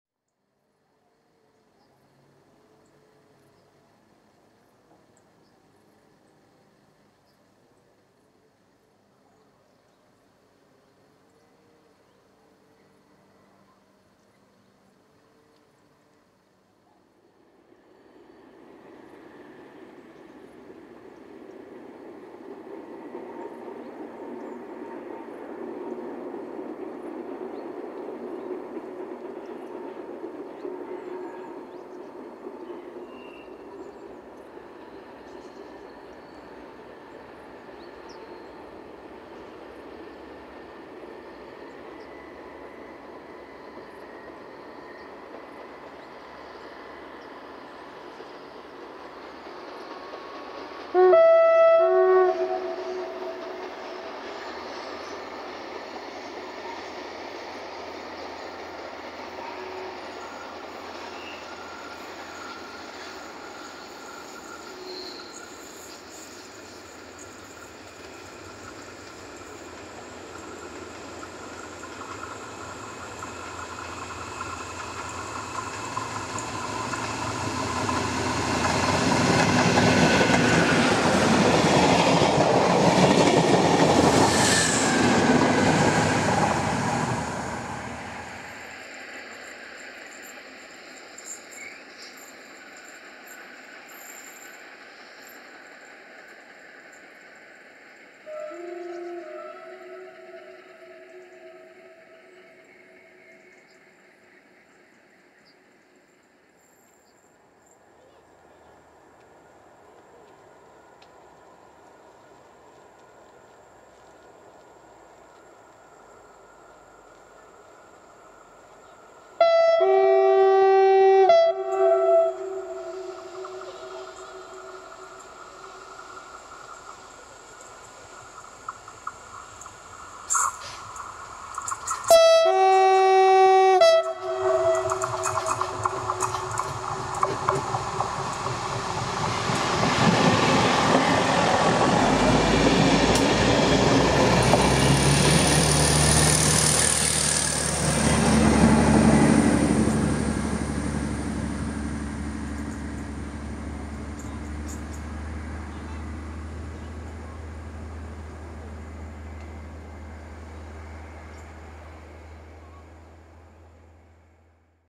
SN84 - Mucha i Strapa w letnie wieczory na linii 108
Po trzech tygodniach wakacyjnego odpoczynku pora nadrobić zaległości w nagraniach, które wciąż zalegają w mojej szufladzie. W ukazanym materiale przenosimy się do lata 2018. Za kolejnością SN84-002 Strapa oraz SN84-001 Mucha jako łącznik TLK 33125 Małopolska rel. Gdynia Główna/Rzeszów - Zagórz przemierzają malownicze tereny w miejscowości Jedlicze.